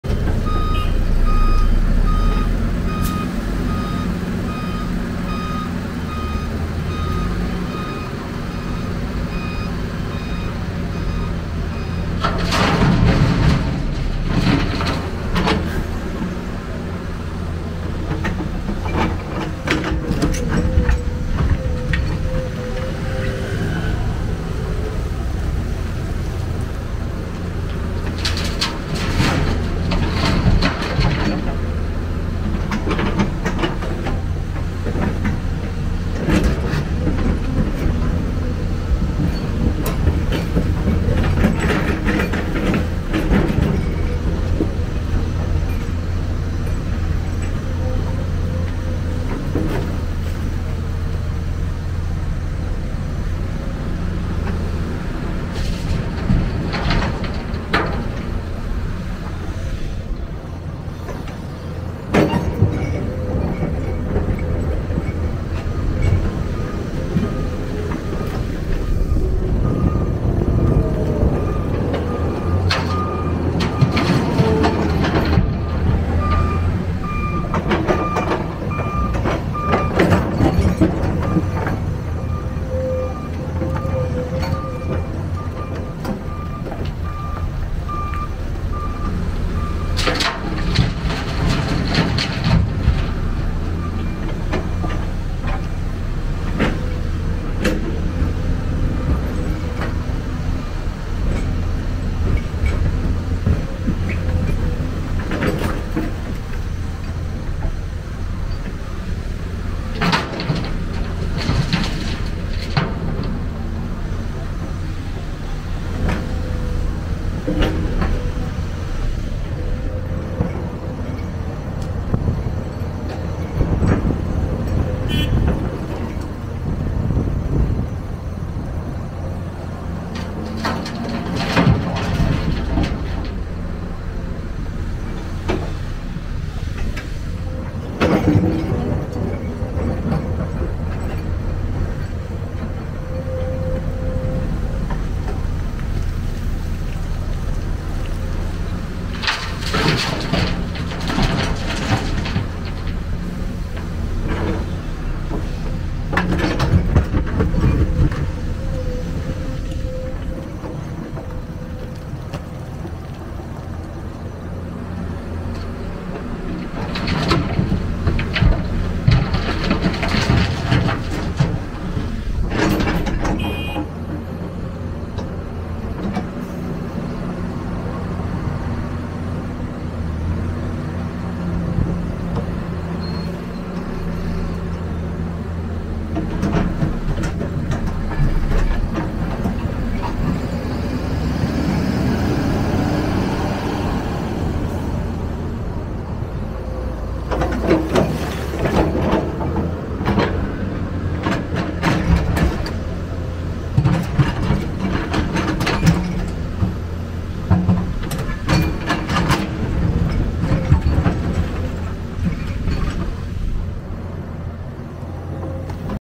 Amazing Excavator Loading Trucks with Incredible Efficiency
This video showcases an incredible excavator efficiently loading trucks with incredible speed. The excavator uses a range of techniques to speed up the process, resulting in an impressive display of efficiency. By watching this video, you can see how the excavator works and how quickly it loads the trucks.